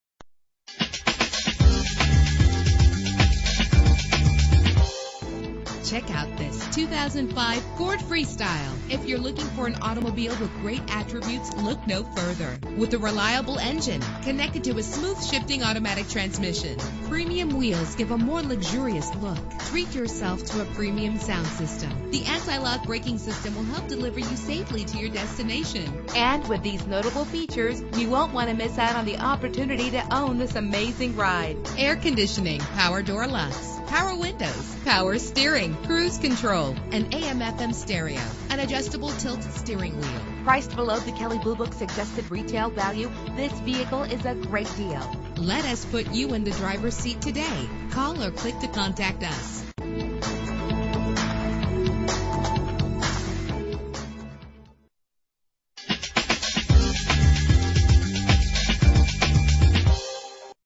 2005 Ford Freestyle - Whitefish MT
Year: 2005
Make: Ford
Model: Freestyle
Trim: Limited Sport Utility 4D
Engine: 6 cylinder 3.0 Liter
Transmission:
Color: Tan
Mileage: 24360
Address:
6331 HWY 93 South